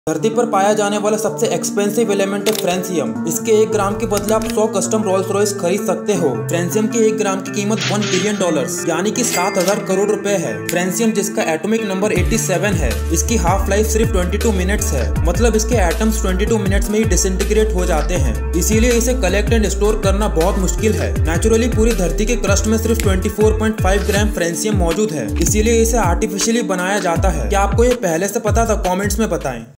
😳 1 Billion Dollars For 1 gram 🤑
This is the Day 42 Of 100 Day 100 Shorts Challenge!!

Francium is a highly reactive and rare alkali metal, belonging to Group 1 of the periodic table. It is represented by the atomic symbol "Fr" and has an atomic number of 87. Francium is radioactive, and its most stable isotope, francium-223, has a very short half-life. Due to its extreme rarity and radioactivity, the properties of francium are challenging to study in detail, and its behavior is often inferred from its neighboring alkali metals. The element was discovered in 1939 by Marguerite Perey, who named it after her home country, France. Because of its scarcity and intense radioactivity, practical applications for francium are limited, and it is primarily of interest to researchers studying the behavior of alkali metals and the fundamental properties of elements.

shorts#ytshorts#francium#earth#expensive#rare#rollsroyce